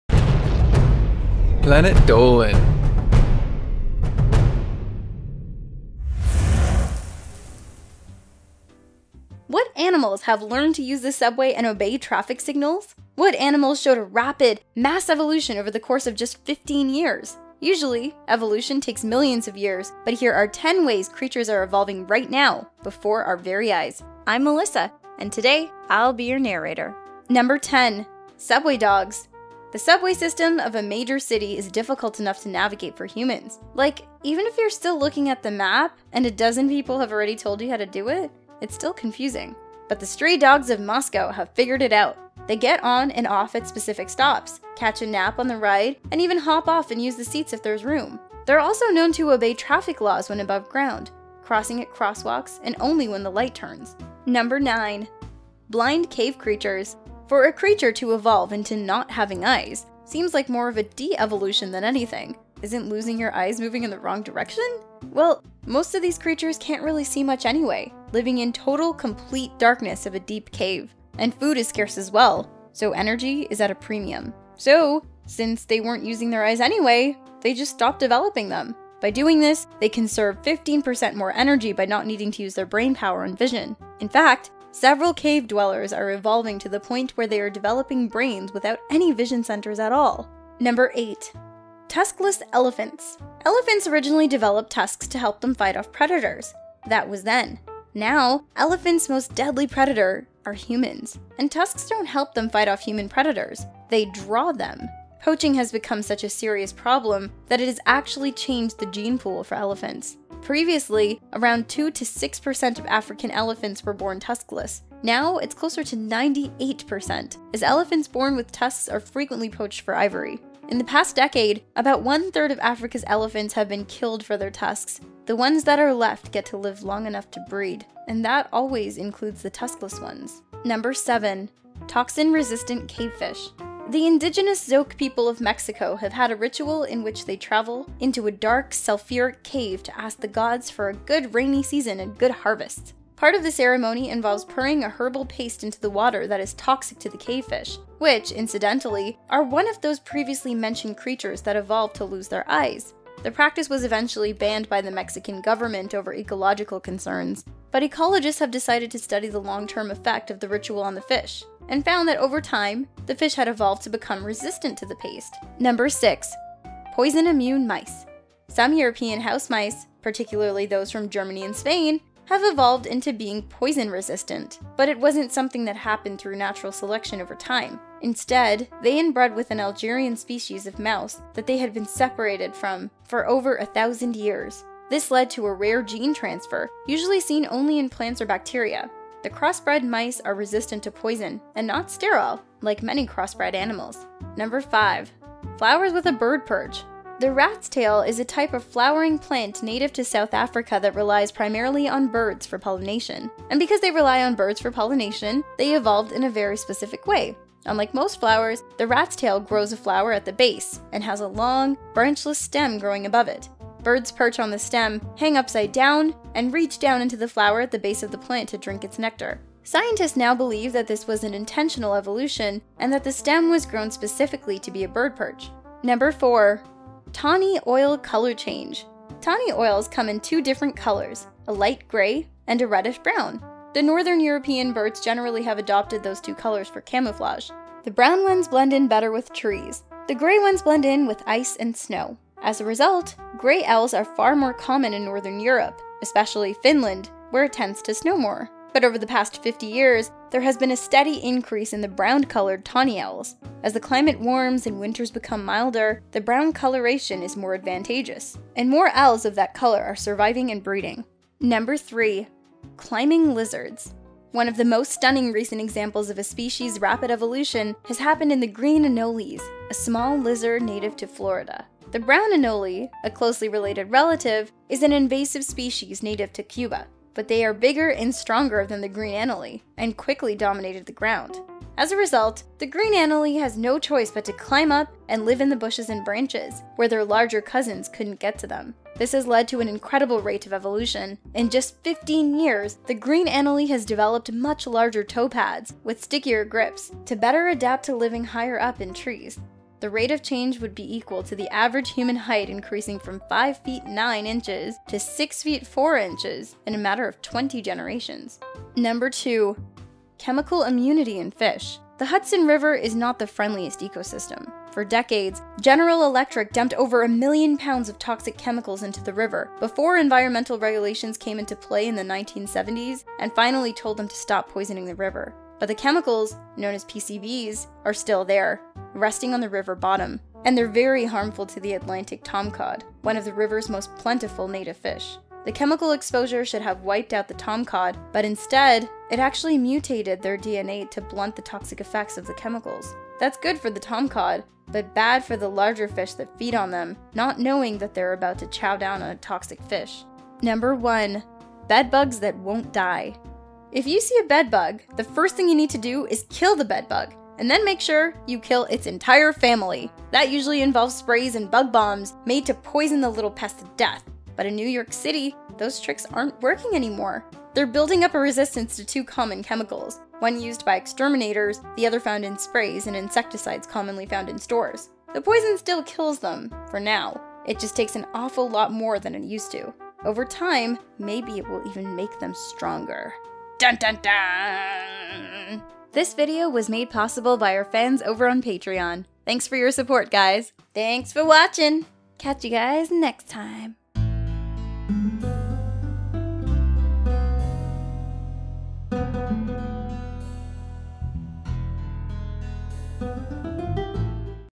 10 Creatures That Are Evolving Before Our Eyes
What animals have learned to use the subway and obey traffic signals? What animals showed a rapid, mass evolution over the course of just 15 years? Usually evolution takes millions of years, but here are 10 ways creatures are evolving right now, before our very eyes